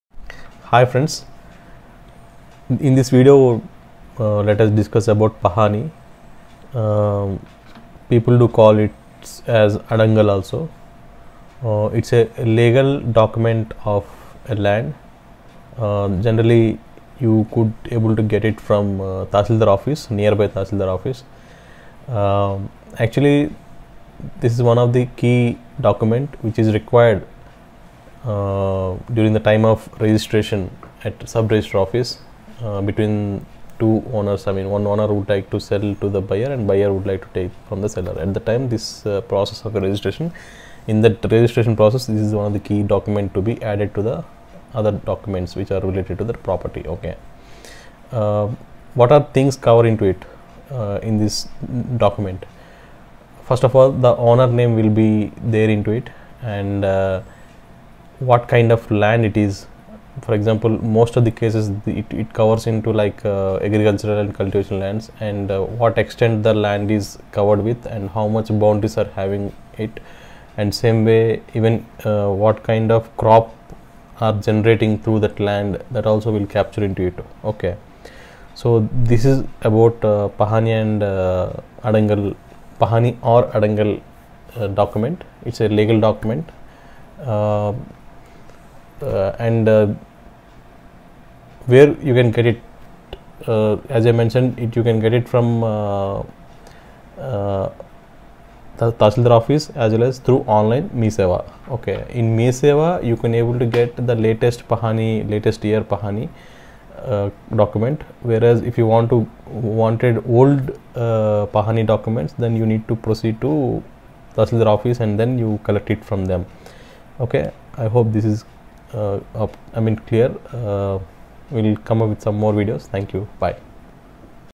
Waht is Pahani or Adangal???
You can understand the meaning Pahani or Adangal document and where you can get the same.